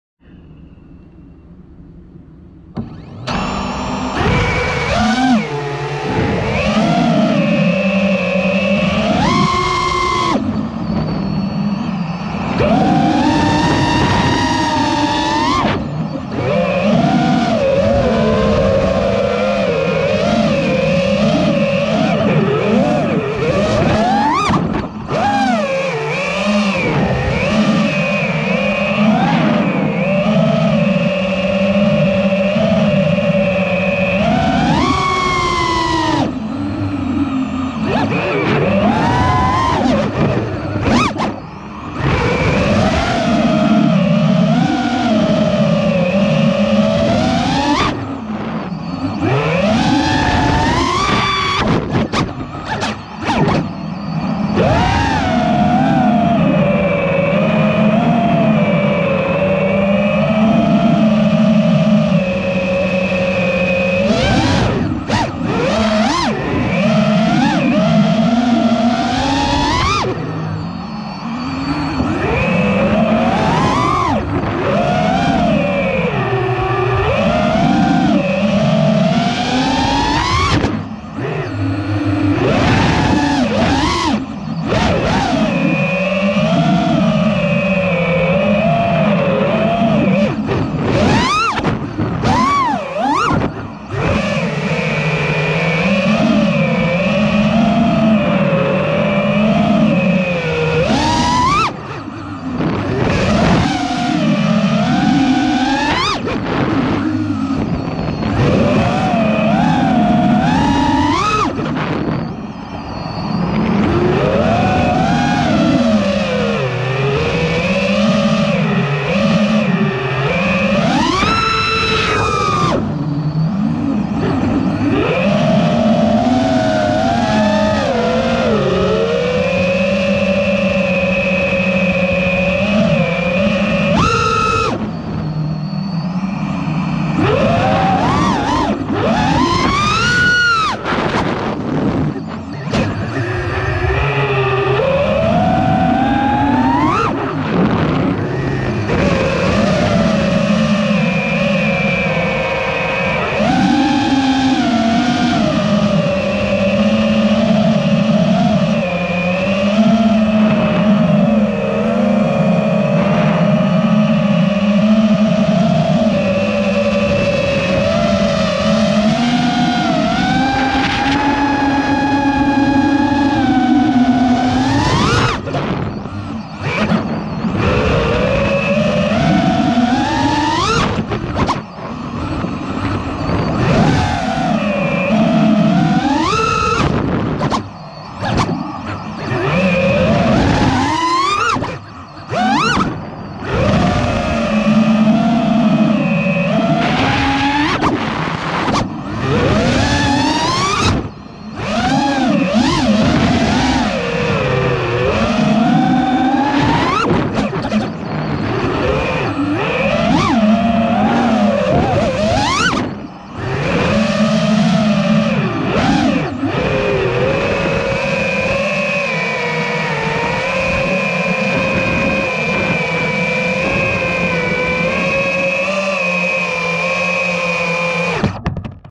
Fettec Reverb maiden
Full fettec Reverb

No twitches or desyncs (from the beginning)

Fettec fc 1.3rc45y
Fettec 45a esc Fw 1.0.155
Fettec osd
Fettec spike absorber
Fettec raceleds
Tmotor F80 PRO 2200kv
TBS Crsf nanorx
TBS Unify pro_32 nano
TBS Triumph pro
Hq prop 5x4,3x3 v2s
Foxeer Predator v4 mini
Dinogy Graphene 1300mah 65c 6s